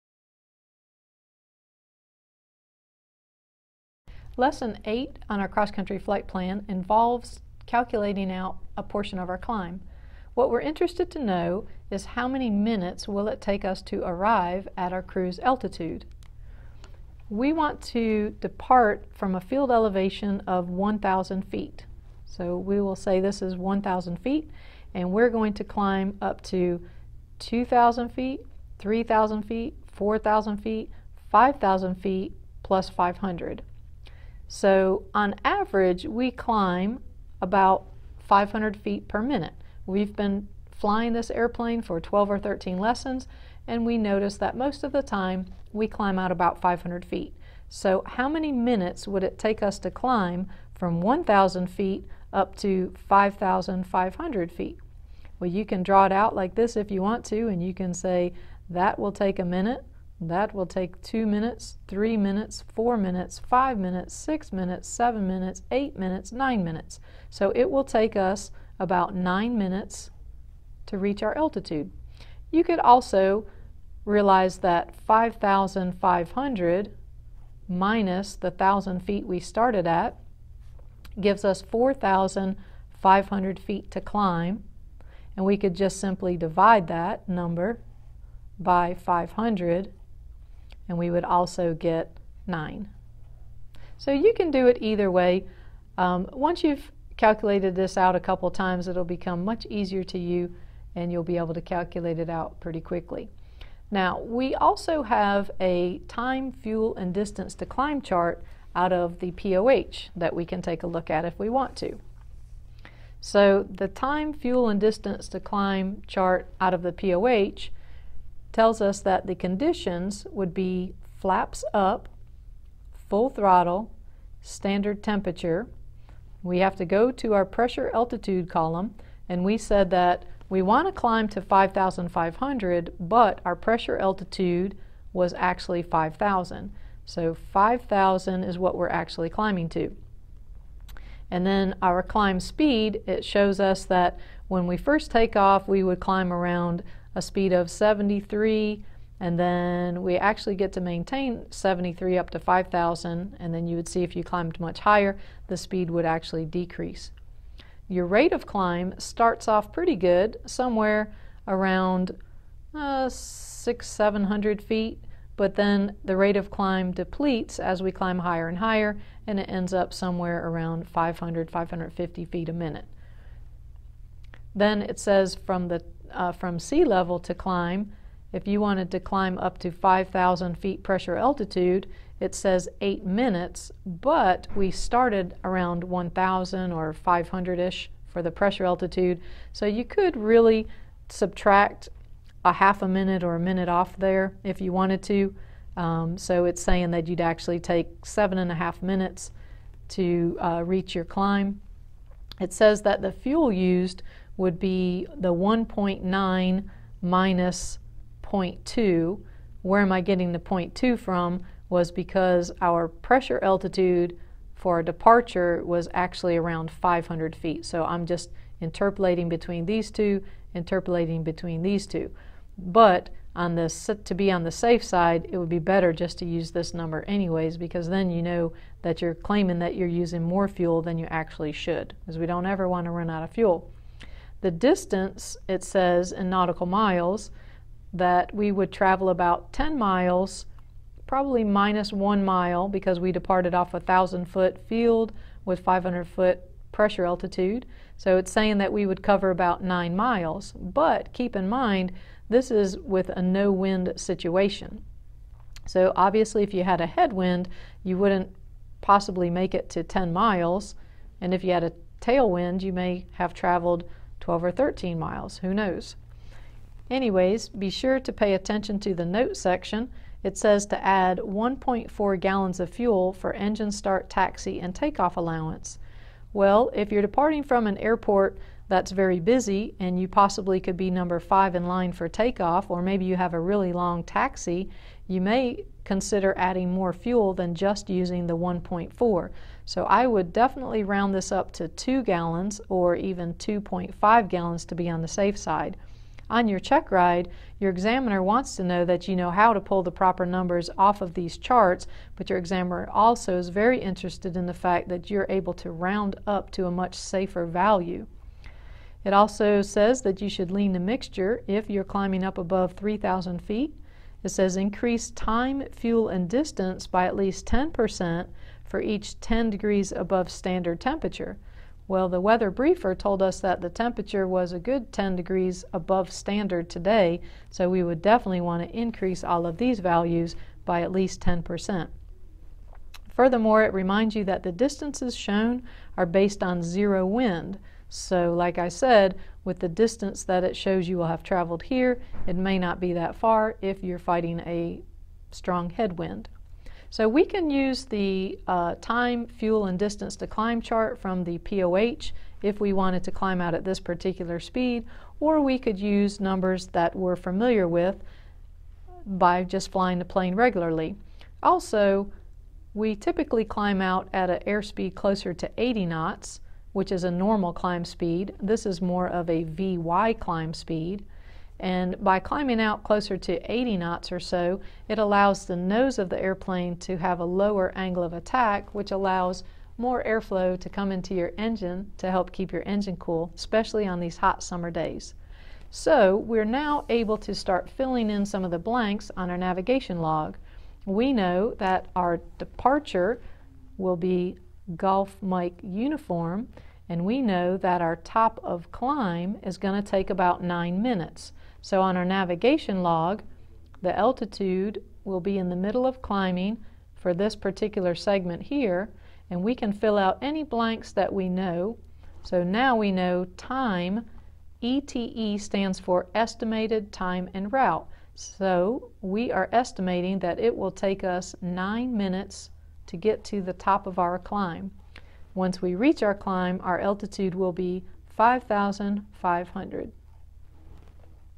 Calculate Top of Climb - XC Flight Planning (Private Pilot Lesson 14h)
Explanation of how to calculate the location of the top of climb for cross country flight planning.

Thinking of becoming a pilot?